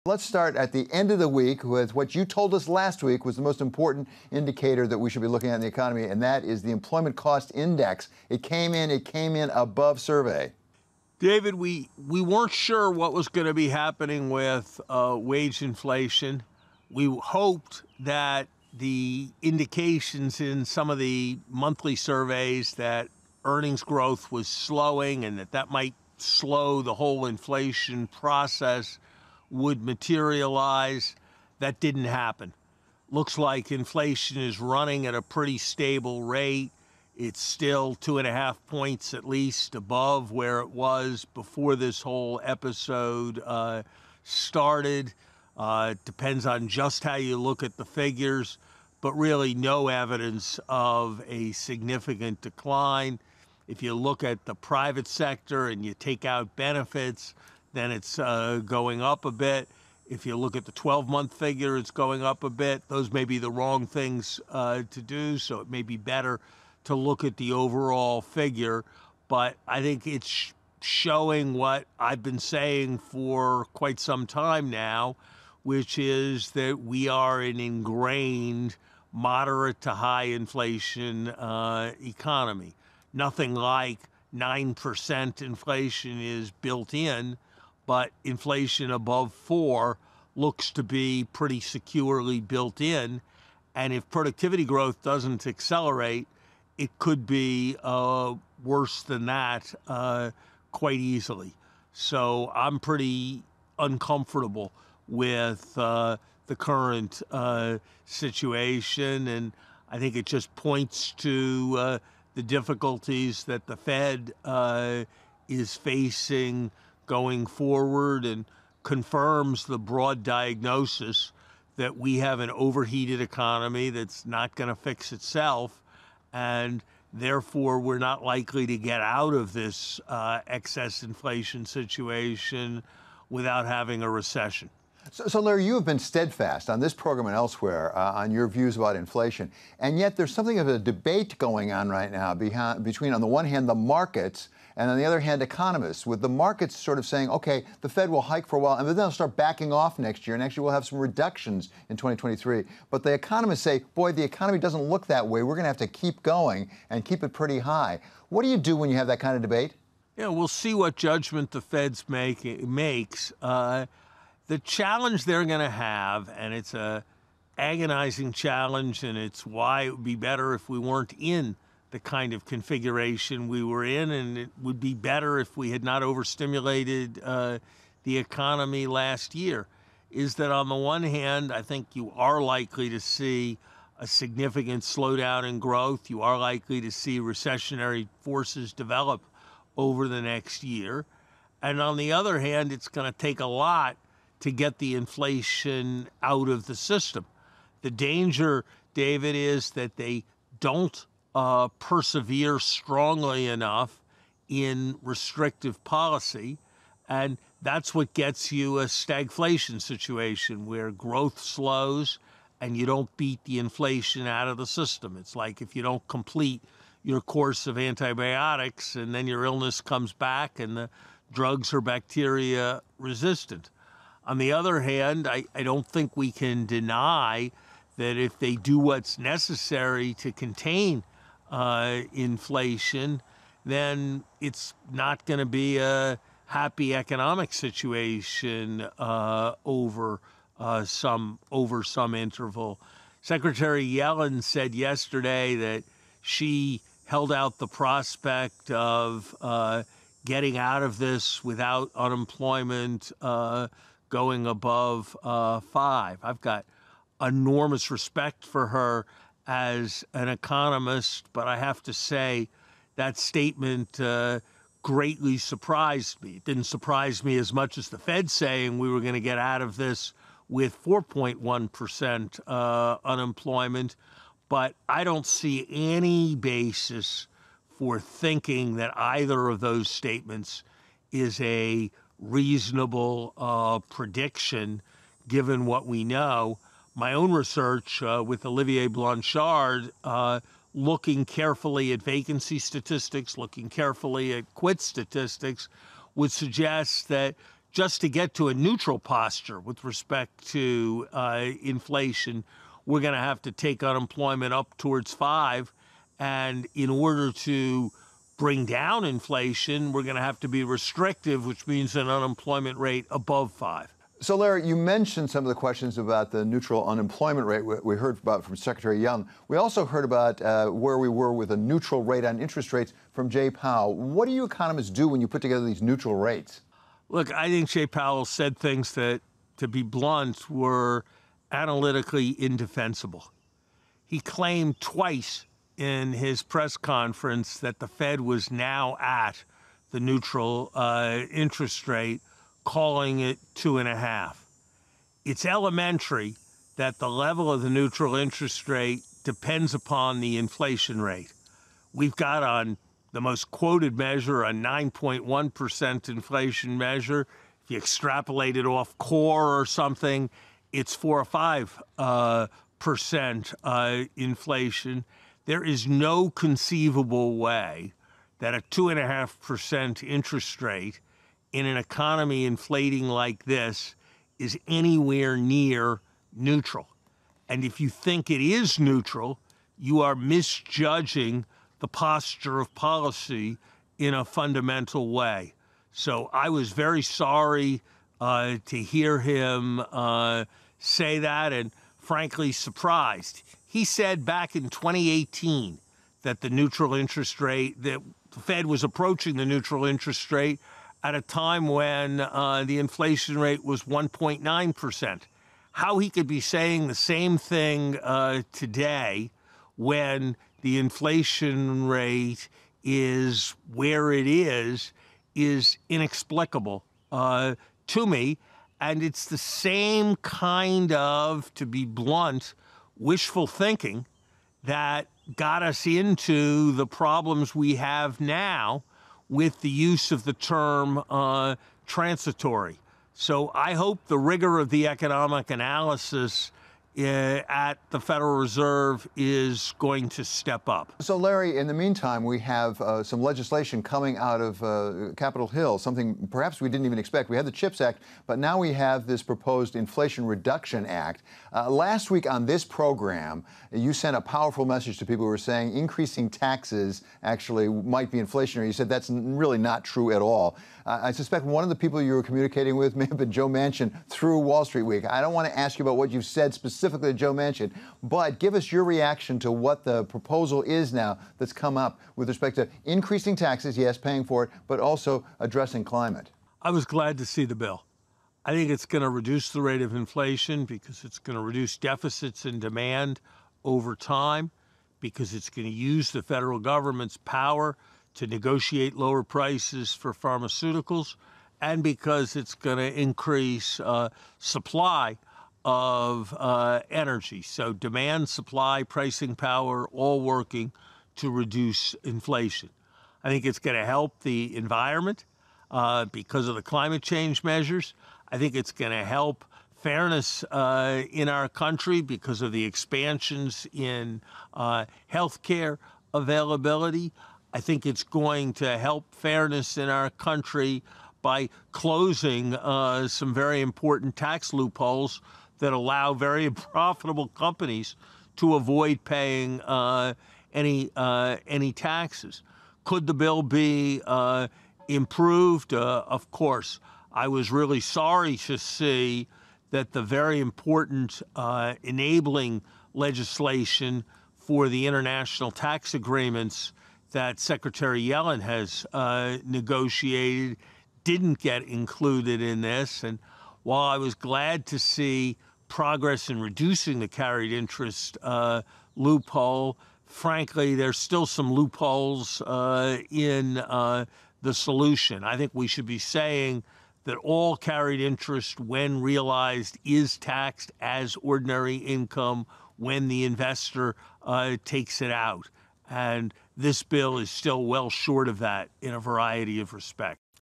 Larry Summers Scorches the Fed and Jay Powell
Former US Treasury Secretary Lawrence H. Summers explains why he believes Powell's call on the neutral fed rate was 'indefensible,' and weighs in on the merits of the Inflation Reduction Act.